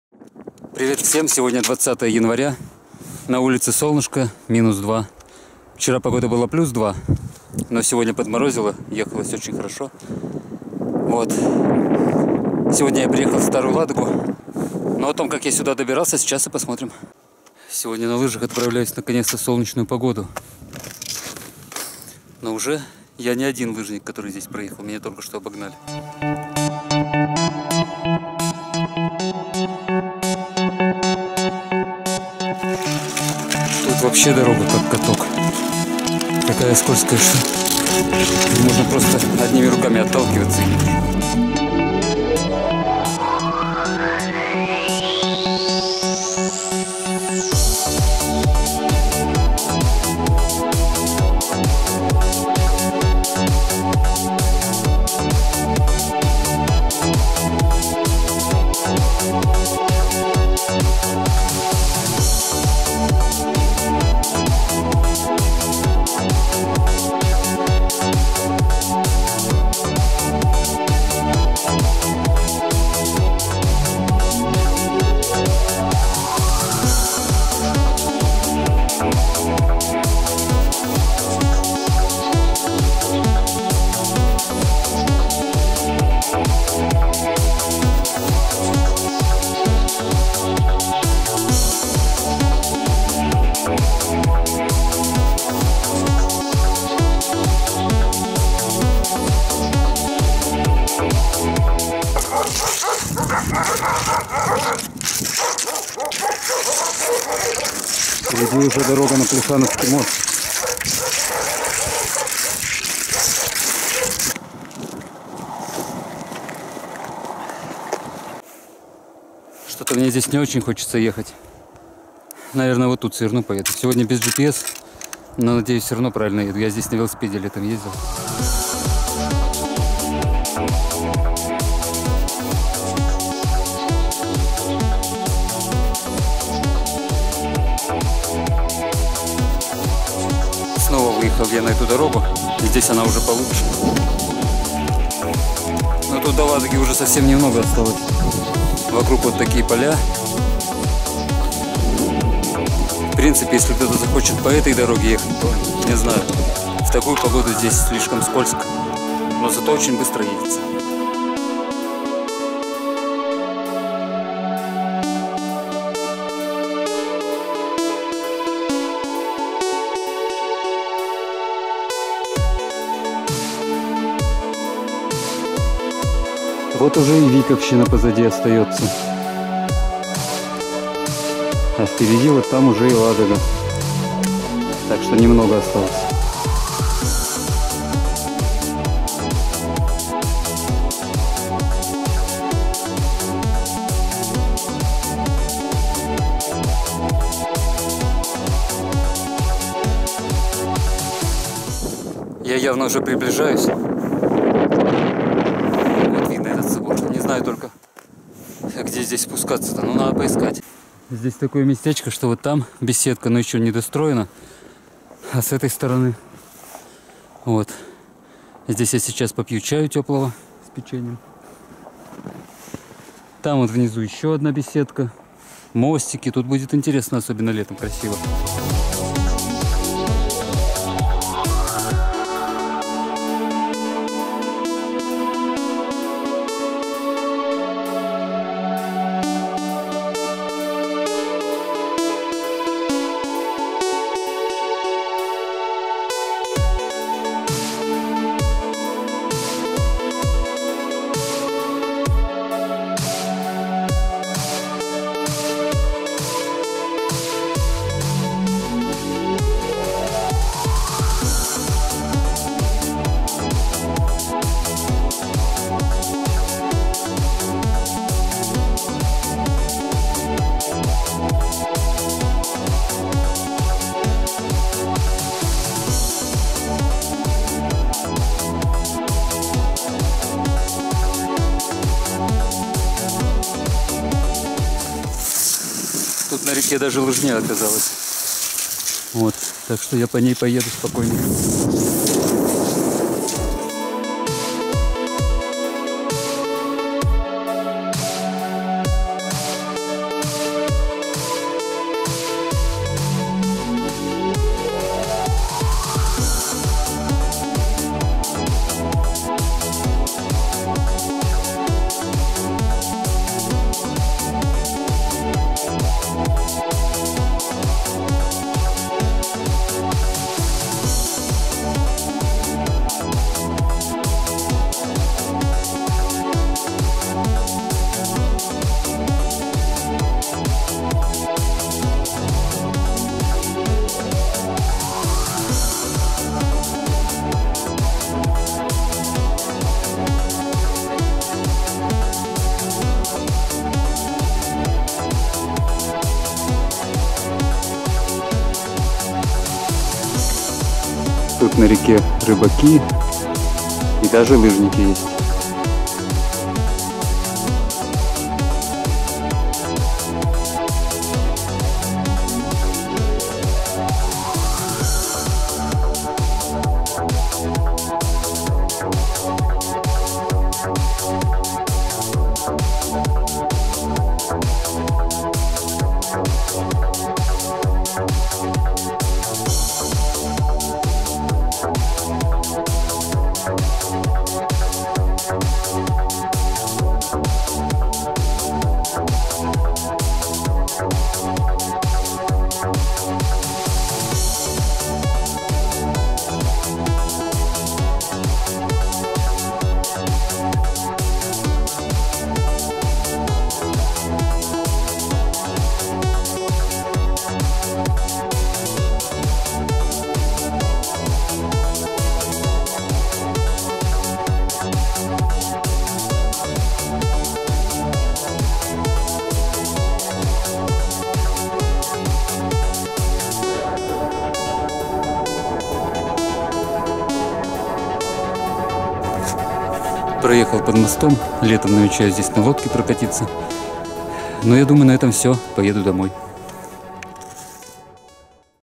из Волхова до Старой Ладоги на лыжах
наконец-то лыжная прогулка в солнечную погоду из Волхова в Старую Ладогу